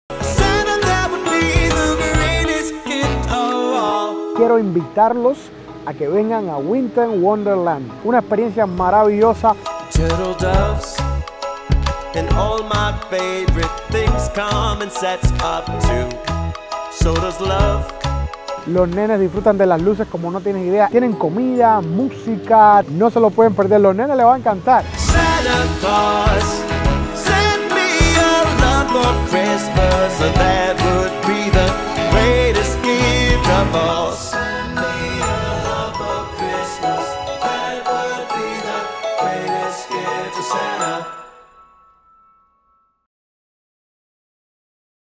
¿¡Que estas esperando!?
¡La alegría navideña y toda la magia de la temporada están aquí en Winter Wonderland Gainesville!

Estamos abiertos todos los viernes y sábados, del 17 de noviembre al 31 de diciembre.
Los boletos de admisión general incluyen:
- Espectáculos 🎄
- Tutorial ligero 💡
- Conoce y saluda a los personajes 🧝‍♀️
- Haz un adorno 🌟
- Decoración de galletas 🍪
- Enviar una carta a Santa ✉️
- Ataque de muñeco de nieve ☃️
- Operaciones fotográficas 📷
* *más actividades y atracciones están disponibles por una tarifa adicional